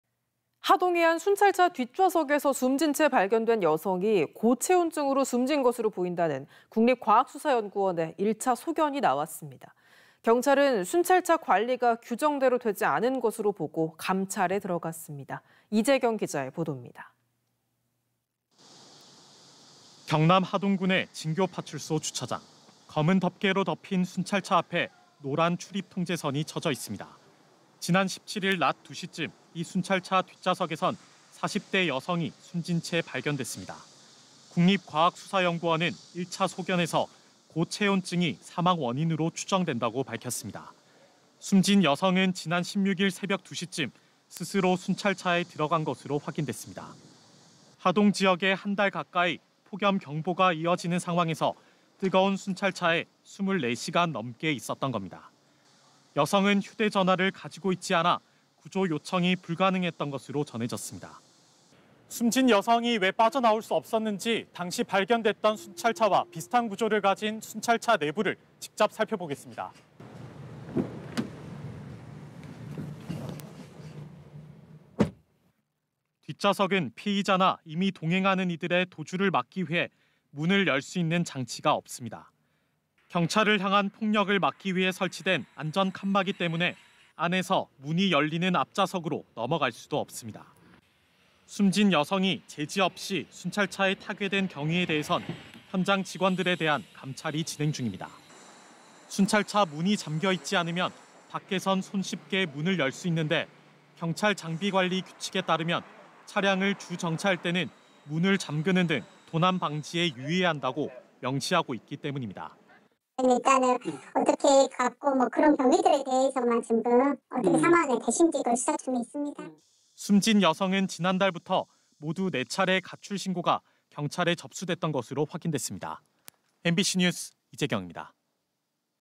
순찰차서 숨진 여성 "고체온증 추정"(2024.8.19/뉴스데스크/MBC경남)
◀ 앵 커 ▶

경남 하동의 한 순찰차 뒷좌석에서
숨진 채 발견된 여성이 고체온증으로
숨졌을 것으로 보인다는
국립과학수사연구원의 1차 소견이 나왔습니다.

경찰은 순찰차 관리가 규정대로
되지 않은 곳으로 보고 감찰에 들어갔습니다.

이재경 기자..

◀ 리포트 ▶

경남 하동군의 진교 파출소 주차장.

검은 덮개로 덮인 순찰차 앞에
노란 출입 통제선이 처져 있습니다.

지난 17일 낮 2시쯤 이 순찰차 뒷좌석에선
40대 여성이 숨진 채 발견됐습니다.

국립과학수사연구원은 1차 소견에서
고체온증이 사망 원인으로 추정된다고
밝혔습니다.

숨진 여성은 지난 16일 새벽 2시쯤 스스로
순찰차에 들어간 것으로 확인됐습니다.

하동 지역에 한 달 가까이
폭염경보가 이어지는 상황에서
뜨거운 순찰차에 24시간 넘게 있었던 겁니다.

여성은 휴대전화를 가지고 있지 않아
구조 요청이 불가능했던 것으로 전해졌습니다.

◀ st-up ▶
\"숨진 여성이 왜 빠져나올 수 없었는지
당시 발견됐던 순찰차와 비슷한 구조를 가진
순찰차 내부를 직접 살펴보겠습니다.\"

뒷좌석은 피의자나 임의 동행하는 이들의
도주를 막기 위해 문을 열 수 있는 장치가
없습니다.

경찰을 향한 폭력을 막기 위해 설치된
안전 칸막이 때문에, 안에서 문이 열리는
앞좌석으로 넘어갈 수도 없습니다.

숨진 여성이 제지 없이 순찰차에
타게 된 경위에 대해선 현장 직원들에 대한
감찰이 진행 중입니다.

순찰차 문이 잠겨 있지 않으면
밖에선 손쉽게 문을 열 수 있는데,

(투명수퍼)
경찰장비관리규칙에 따르면
\"차량을 주·정차할 때는 문을 잠그는 등
도난방지에 유의해야 한다\"고
명시하고 있기 때문입니다.//

◀ SYNC ▶경찰 관계자(음성 변조)
\"일단은 어떻게 갔고 그런 경위들에 대해서만, 지금 어떻게 사망하게 되신지 그걸 수사 중에 있습니다.\"

숨진 여성은 지난달부터
모두 4차례 가출신고가 경찰에
접수됐던 것으로 확인됐습니다.

MBC 뉴스 이재경입니다.

◀ END ▶